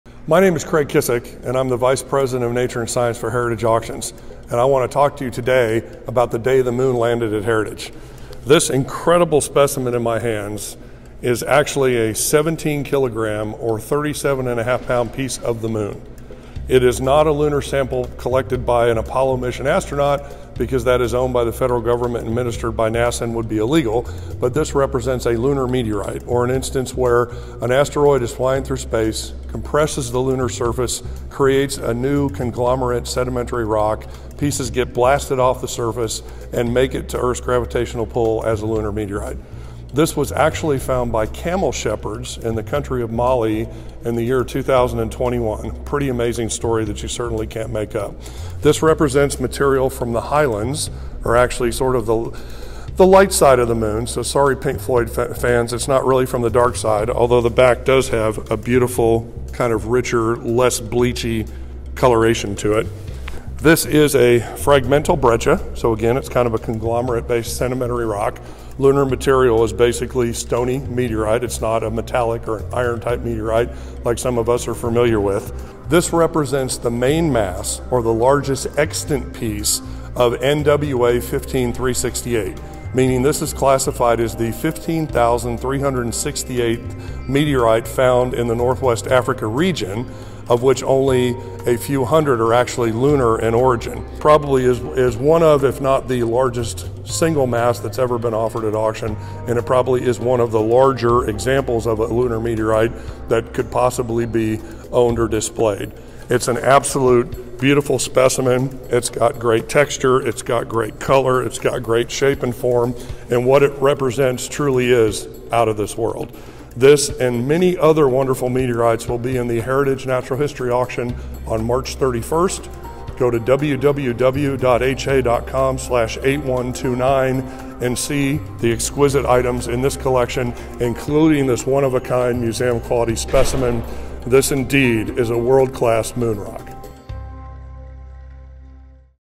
37-Pound Lunar Meteorite, One of the Largest to Come to Auction, Touches Down at Heritage Auctions
A massive NWA 15368 Lunar Meteorite is offered March 31 in Heritage Auctions’ Nature & Science Signature® Auction.

The offered lunar meteorite is massive — almost the size of a basketball and weighing in at nearly 17 kilograms (37.42 pounds). It was found in the north African nation of Mali, by a couple of camel shepherds who came across it in the Sahara Desert in January 2021.

The highly textural surfaces of this giant meteorite exhibit mottled coloration, shallow planetary crater indentations, and a contrast of light and dark between the primary and secondary display face.